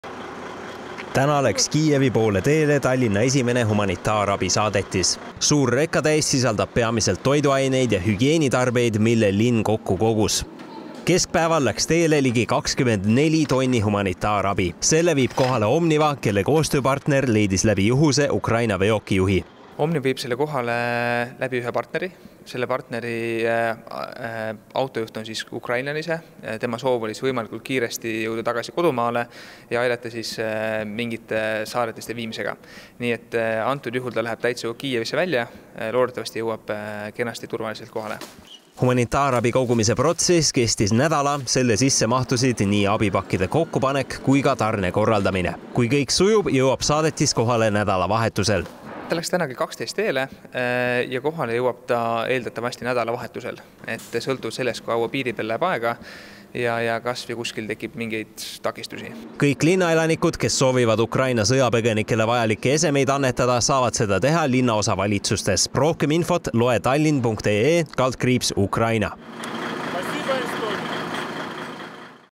15.03.2022 - Vaata, mis oli Tallinna humanitaarabisaadetises Ukrainale
Omniva logistikakeskusest startis täna Tallinna linna poolt Kiievisse teele järelhaagisega reka, mis on täis toidu- ja esmatarbekaupa kohalikele elanikele. Veoauto roolis on Ukraina autojuht, kes lubas pärast saadetise kohaletoimetamist jääda Kiievisse oma riiki kaitsma.